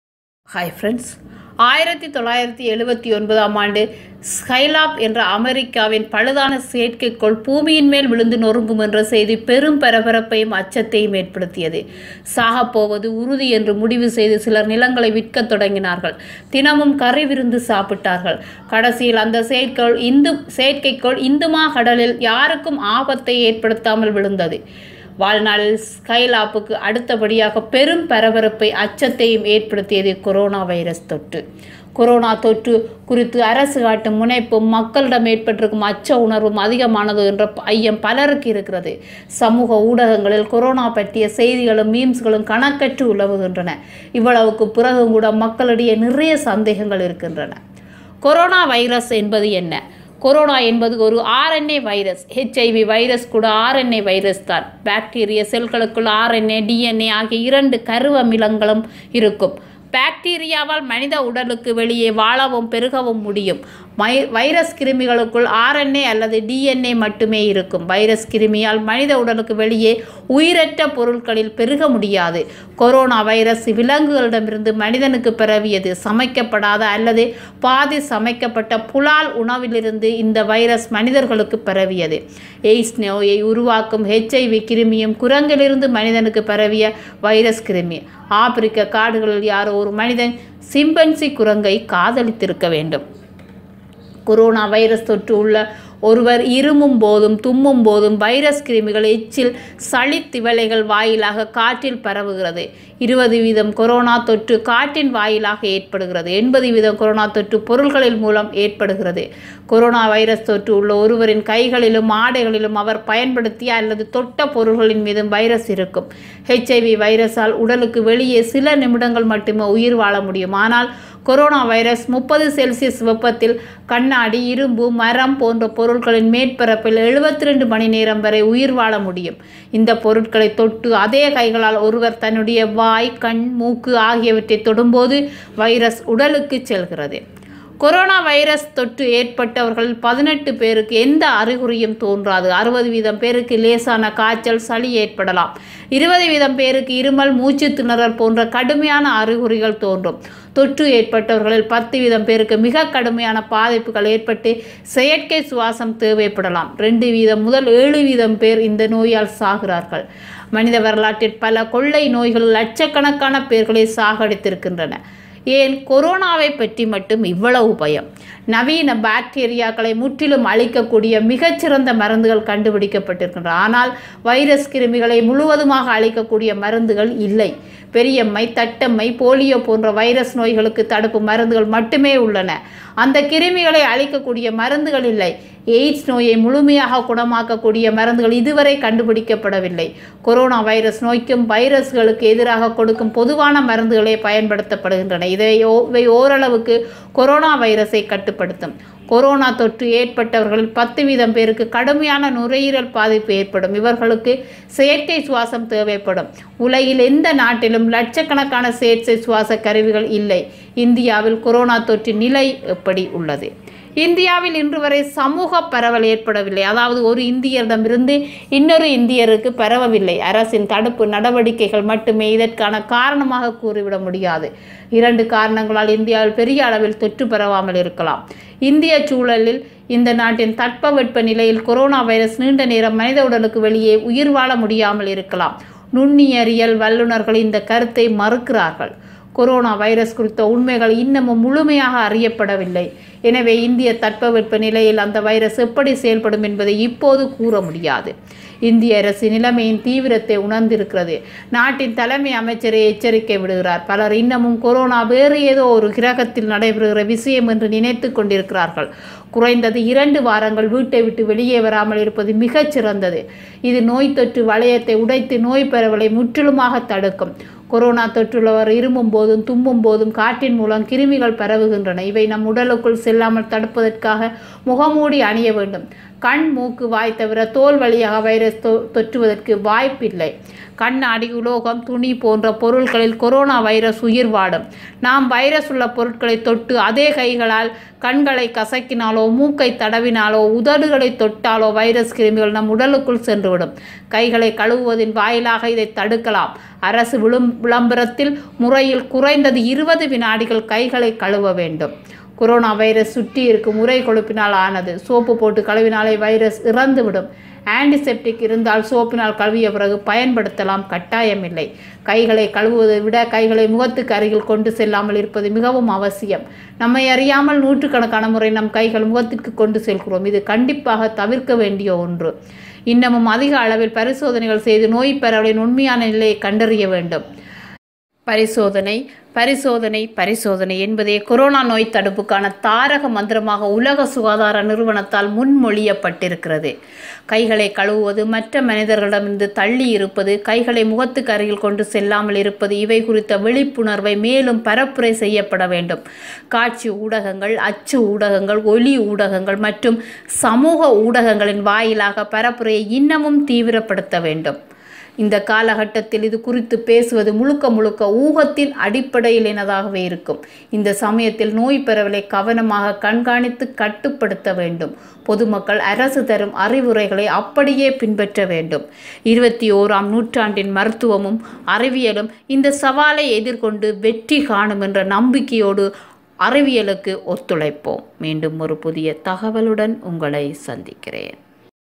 ஏன் கொரோனாவைப் பற்றி மட்டும் இவ்வளவு பயம்
கொரோனா என்பது ஒரு RNA வைரஸ்